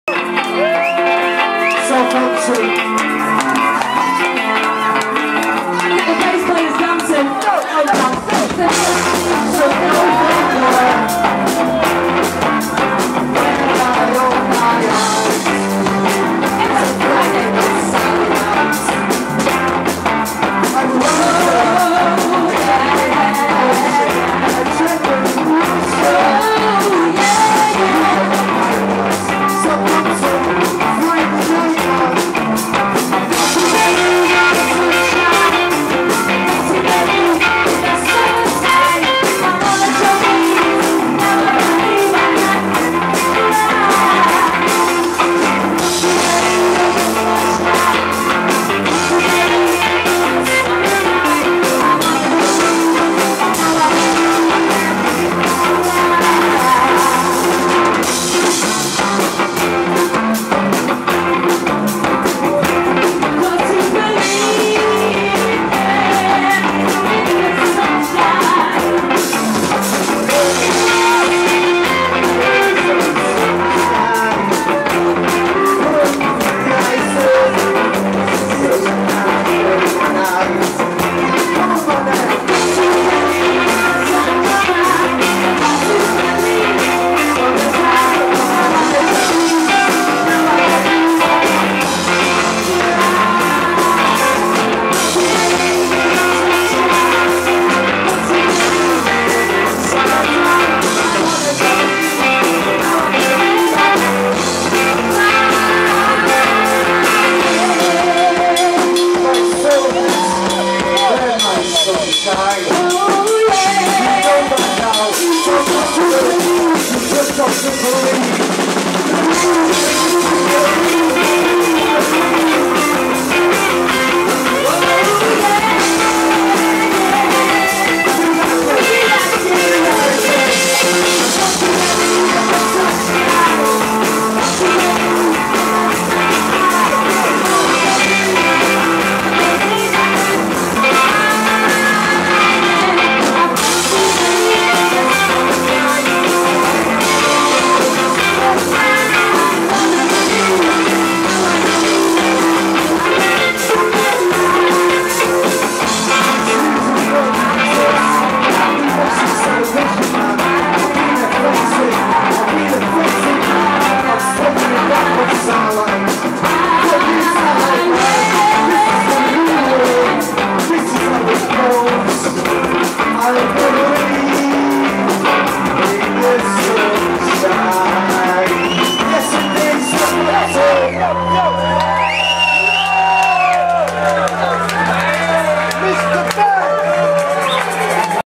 Bez & Domino Bones
Happy Monday's legend Bez and his awesome band play The Soul Cellar in Southampton